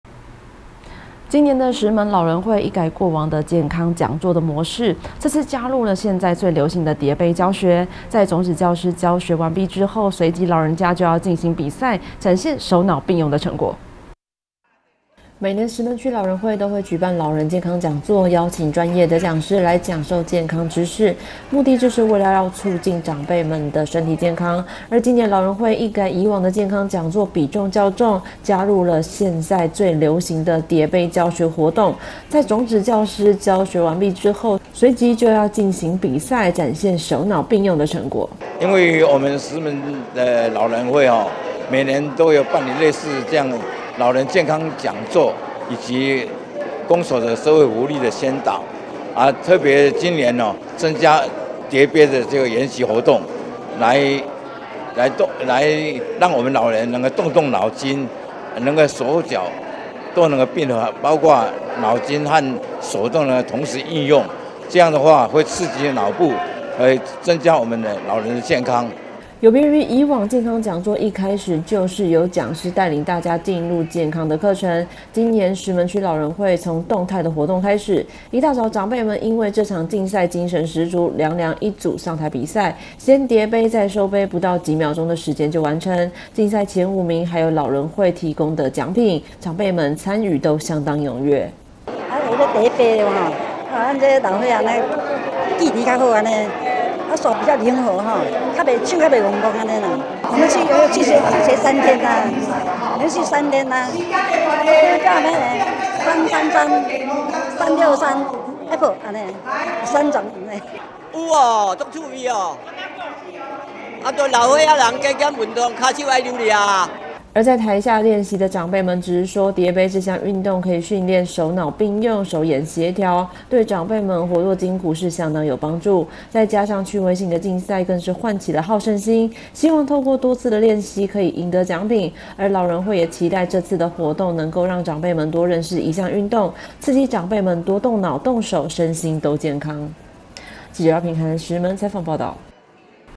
石門老人會健康講座 疊杯競賽好刺激
每年石門區老人會都會舉辦老人健康講座，邀請專業的講師來講授健康的知識，目的就是為了要促進長輩們的身體健康，今年老人會一改過往的健康講座比重較重，加入了現在做流行的疊杯教學活動，在種子教師教學完畢之後，隨即就要進行比賽，展現手腦並用的成果。

石門區老人會常務監事李佳安說，有別於以往健康講座一開始就是由講師帶領大家進入健康的課程，今年石門區老人會從動態活動開始，一大早長輩們因為這場競賽精神十足，兩兩一組上台比賽，先疊杯再收杯，不到幾秒鐘的時間就完成，競賽前五名還有老人會提供的獎品，長輩們參與都相當踴躍。

而在台下練習的長輩們直說，疊杯這項運動可以訓練手腦並用、手眼協調，對長輩們活絡筋骨相當有幫助，再加上趣味性的競賽，更是喚起了好勝心，希望透過多次的練習，可以贏得奬品，而老人會也期待這次的活動，能讓長輩們多認識一項運動，刺激長輩多動腦、動手，身心都更健康。